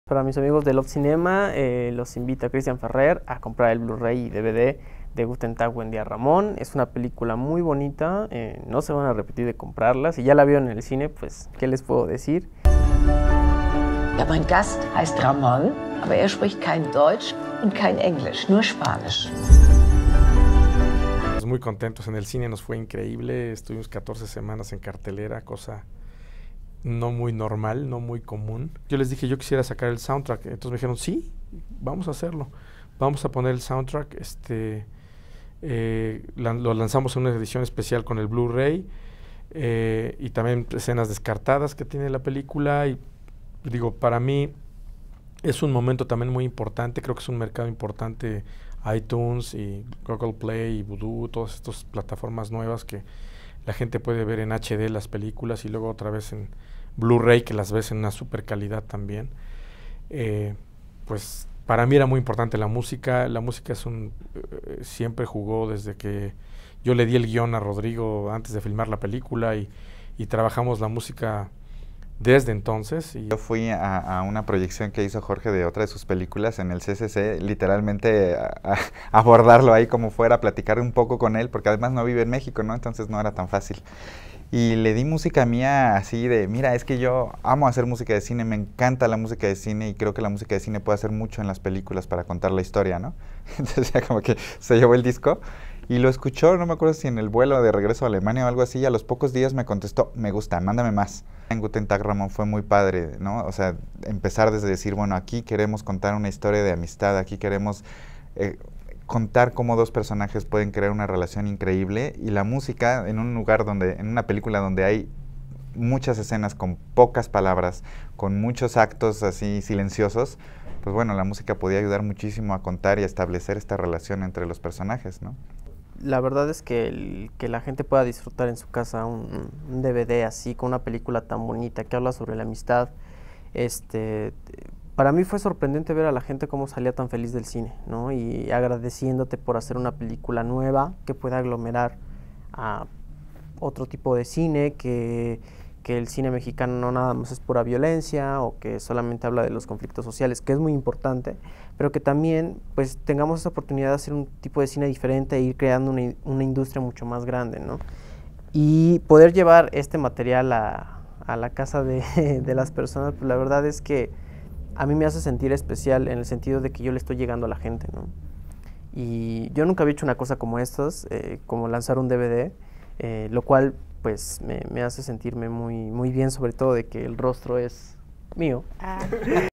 Guten Tag Ramón / Kristyan Ferrer Lanzamiento del DVD - LOFT CINEMA
Entrevistas con el director Jorge Ramírez-Suárez, Protagonista Kristyan Ferrer, y el músico Rodrigo Flores

Guten Tag, Ramón cuenta la historia de Ramón, un joven de una ranchería del norte del país, que se niega a ser delincuente y decide buscar a la tía de un amigo en Alemania, quien le dará trabajo. Pero al llegar nunca encuentra a la tía. Sin dinero, sin papeles y sin hablar otro idioma más que el suyo, sobrevive apenas en las calles hasta que conoce a Ruth, una solitaria anciana jubilada. Ella le ofrece apoyo sin hablar el mismo idioma, se comunican con mímica y dibujos. Utilizando el lenguaje universal de la amistad, ambos la entablan de una forma maravillosa.